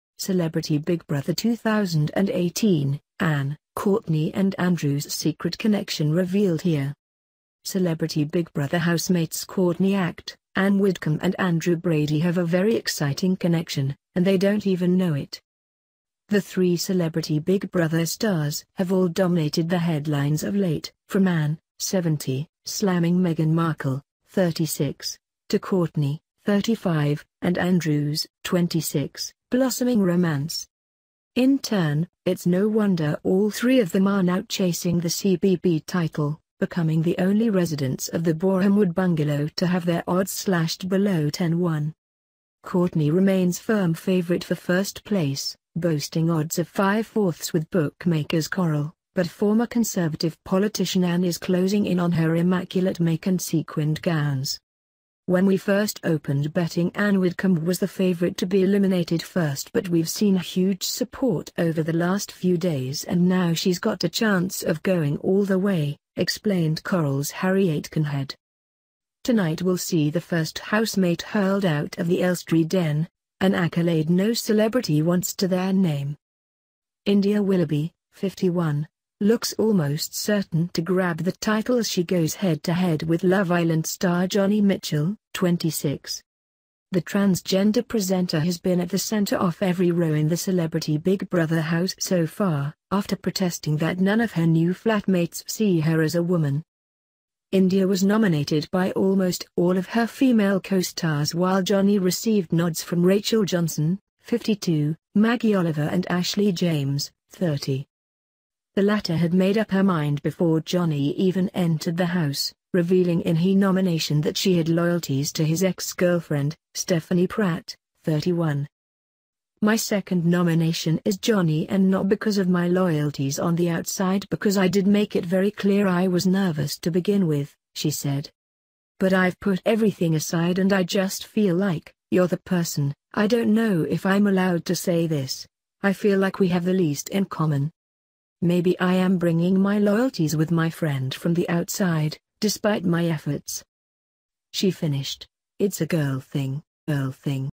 Celebrity Big Brother 2018: Ann, Courtney and Andrew's secret connection revealed HERE
CELEBRITY BIG BROTHER housemates Courtney Act, Ann Widdecombe and Andrew Brady have a very exciting connection - and they don’t even know it.

The three Celebrity Big Brother stars have all dominated the headlines of late, from Ann, 70, slamming Meghan Markle, 36, to Courtney, 35, and Andrew’s, 26, blossoming romance.

In turn, it’s no wonder all three of them are now chasing the CBB title, becoming the only residents of the Borehamwood bungalow to have their odds slashed below 10/1.

Courtney remains firm favourite for first place, boasting odds of 5/4 with bookmakers Coral, but former Conservative politician Ann is closing in on her immaculate make and sequinned gowns.

“When we first opened betting Anne Widdecombe was the favourite to be eliminated first but we’ve seen huge support over the last few days and now she’s got a chance of going all the way,” explained Coral’s Harry Aitkenhead.

Tonight will see the first housemate hurled out of the Elstree den; an accolade no celebrity wants to their name.

India Willoughby, 51, looks almost certain to grab the title as she goes head to head with Love Island star Johnny Mitchell, 26.

The transgender presenter has been at the centre off every row in the Celebrity Big Brother house so far, after protesting that none of her new flatmates see her as a woman.

India was nominated by almost all of her female co-stars while Jonny received nods from Rachel Johnson, 52, Maggie Oliver and Ashley James, 30.

The latter had made up her mind before Johnny even entered the house, revealing in he nomination that she had “loyalties” to his ex-girlfriend, Stephanie Pratt, 31.

“My second nomination is Jonny and not because of my loyalties on the outside because I did make it very clear I was nervous to begin with,” she said.

"But I've put everything aside and I just feel like, you're the person, I don't know if I'm allowed to say this, I feel like we have the least in common.

"Maybe I am bringing my loyalties with my friend from the outside, despite my efforts."

She finished: "It's a girl thing."

Ann Widdecombe now among favourites to win Celebrity Big Brother,
India Willoughby is favourite to leave Celebrity Big Brother tonight,
Ashley James nominated Johnny Mitchell on Celebrity Big Brother,